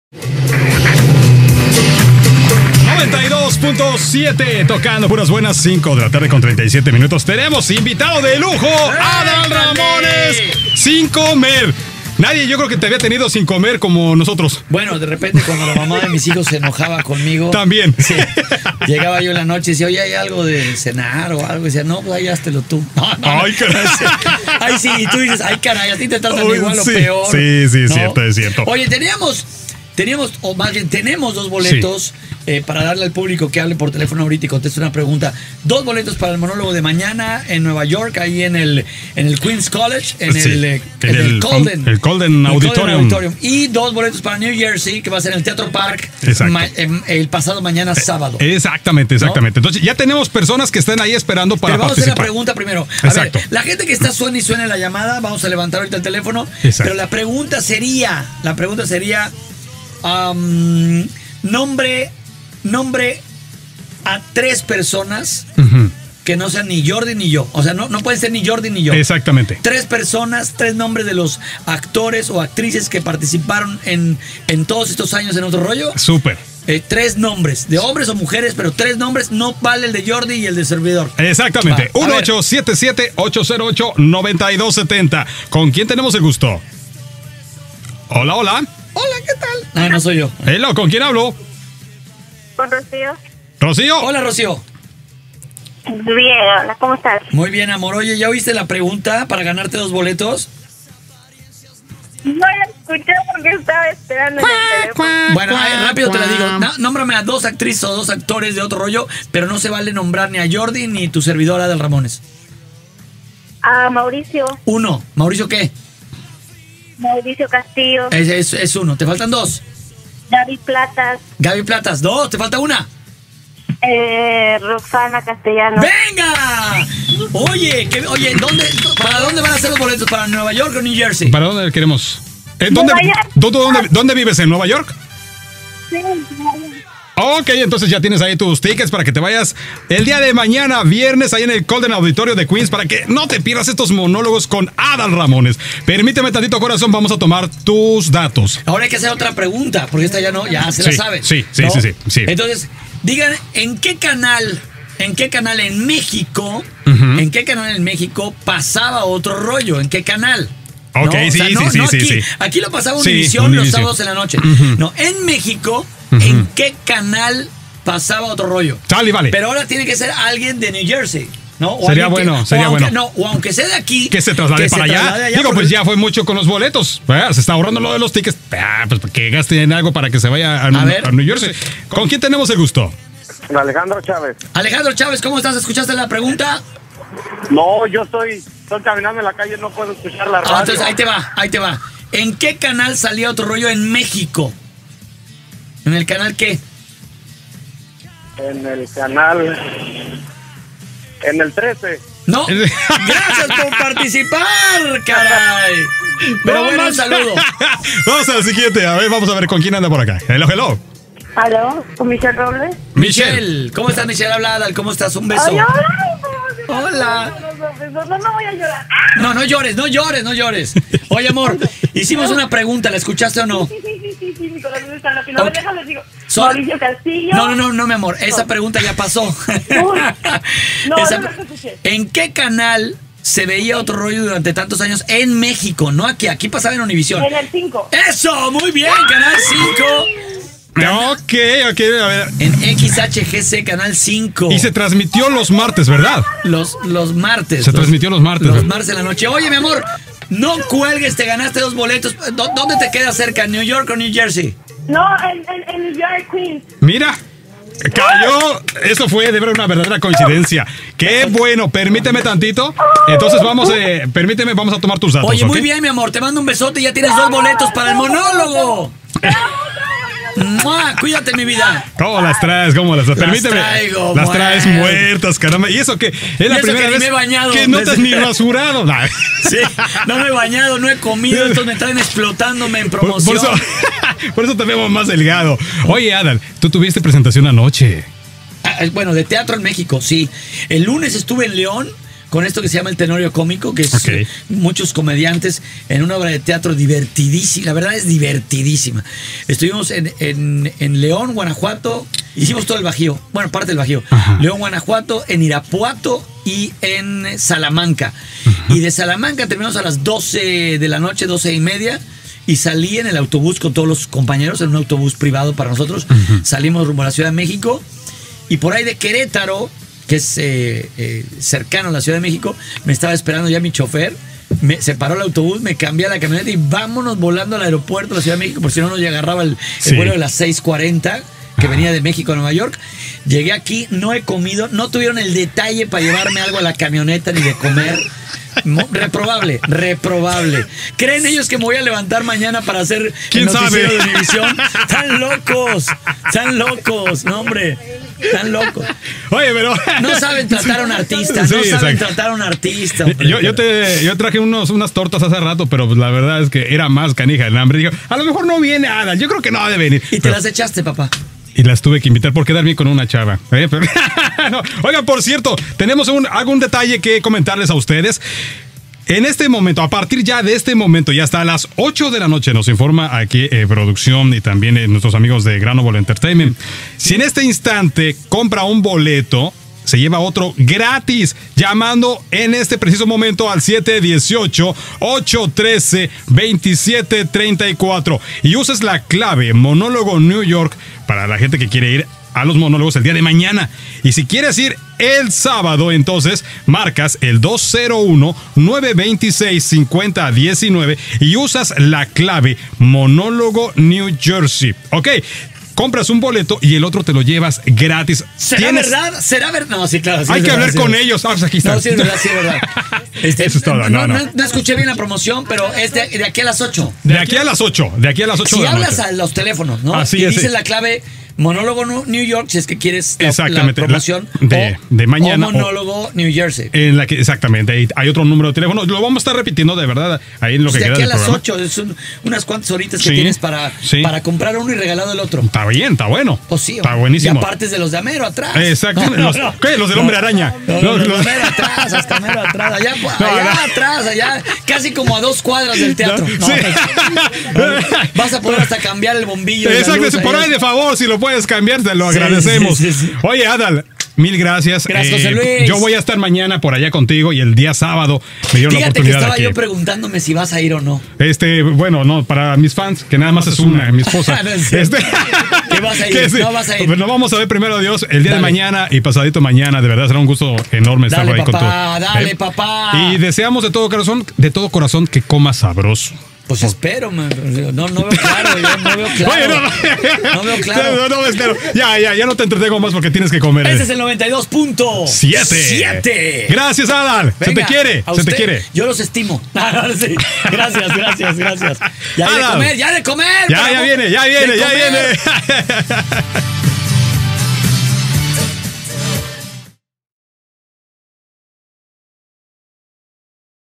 Adal Ramones en 92.7 FM Nueva York
Adal Ramones y sus Monólogos Tour 2015 se llevaron a cabo el 5 y 6 de Junio. Previo a sus presentaciones, un dia antes nos visitó en el estudio de la emisora, la finalidad, invitar a nuestro público a gozar de tan divertido espectáculo.